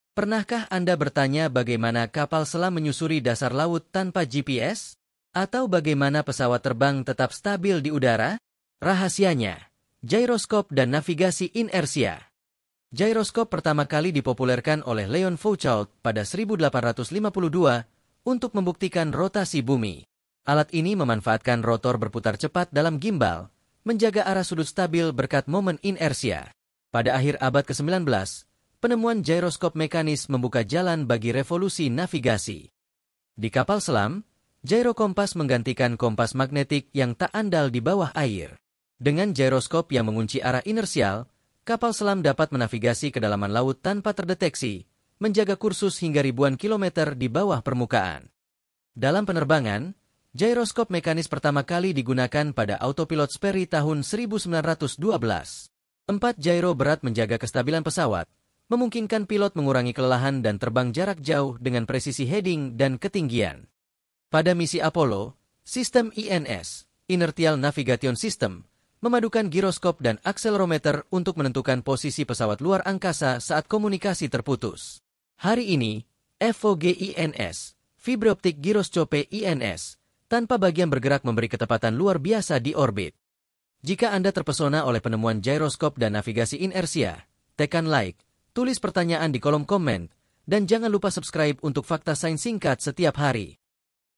Penemuan Gyroskop: Sejarah & Cara Kerja Navigasi Inersia
Pelajari penemuan gyroskop dan prinsip navigasi inersia dari kapal selam hingga luar angkasa